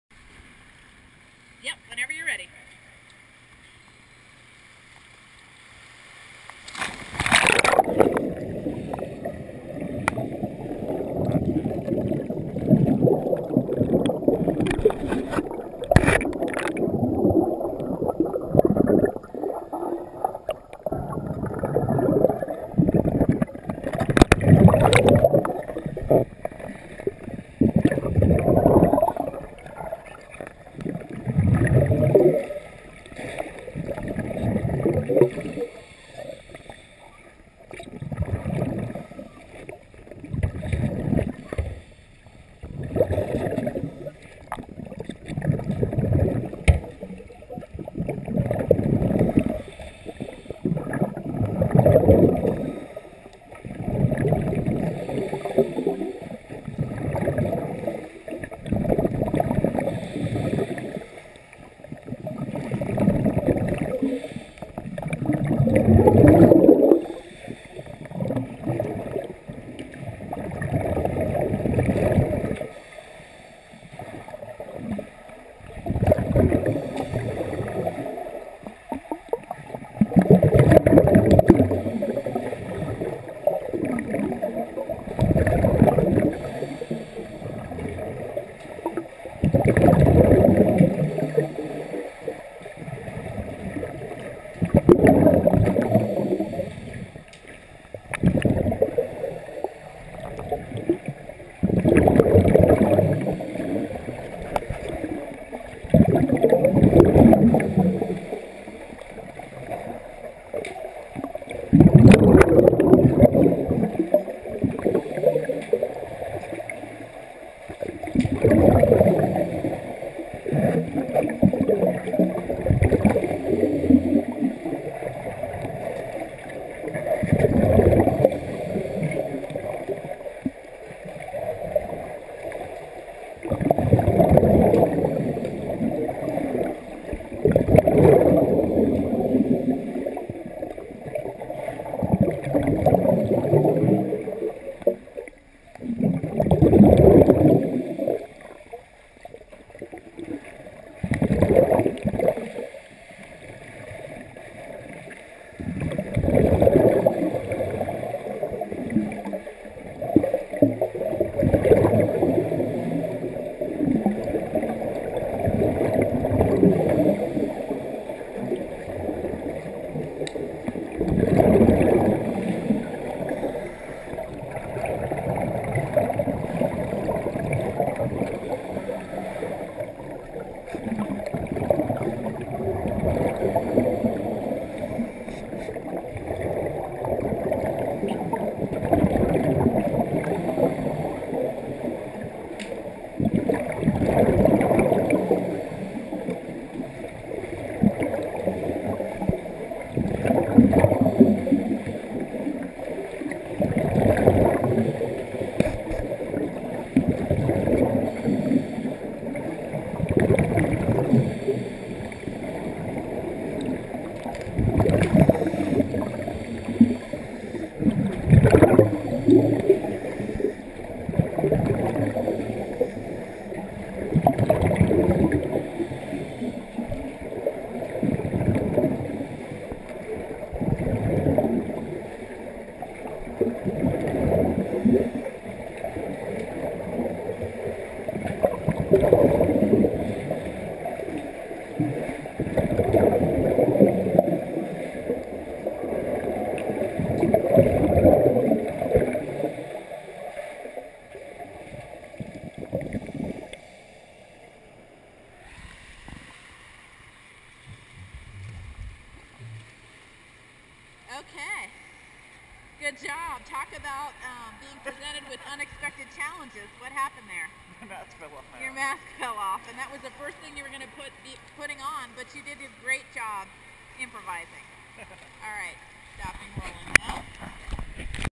Adam's Successful Bail-In in SCUBA Instructor Gabrielle's Class
Adam did great on his bail-in skill for open water SCUBA certification. This is a required skill with SCUBA Educators International (SEI), used to be YMCA, which helps demonstrate excellent problem-solving techniques when faced with obstacles.